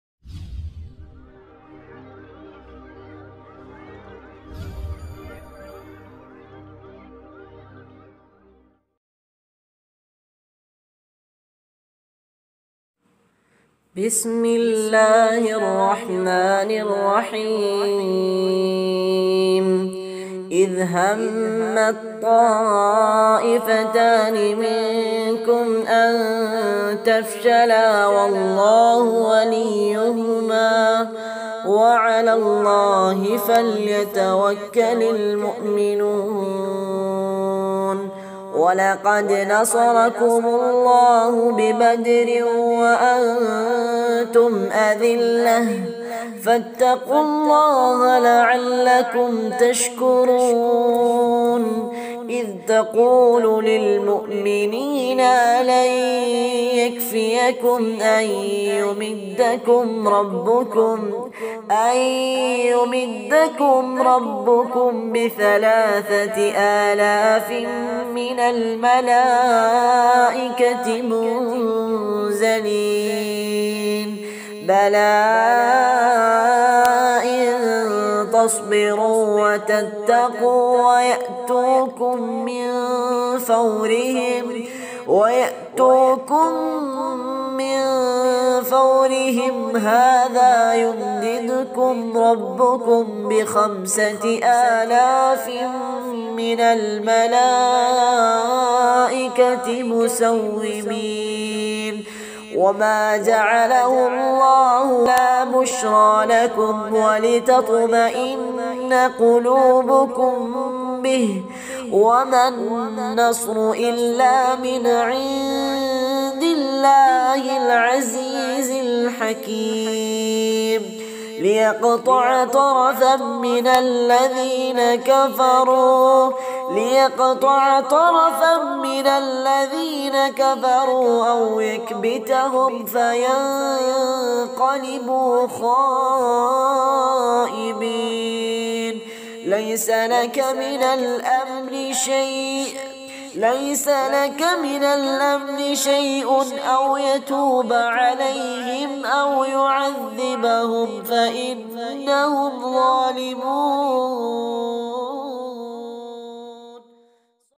EMOTIONAL AND HEART TOUCHING QURAN RECITATION | HAFIZ SAEED ATTOOR
THE REAL BEAUTY OF HOLY QURAN.
WE CAN FLEE TO MAKKAH TO EMBRACE THE HISTORICAL BADR. THROUGH THE FINE VOICE OF HAFIZ SAEED ATTOOR